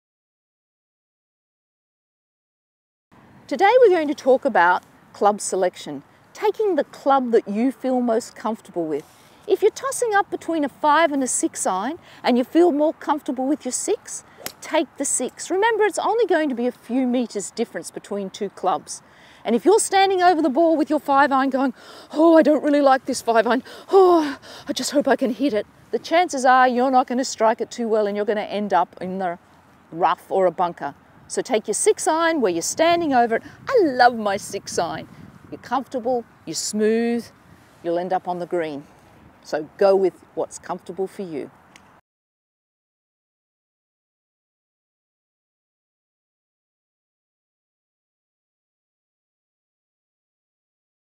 Love your club selection
Todays tip looks at the importance of taking a club that you feel most comfortable with and forgetting about the loss in a few metres.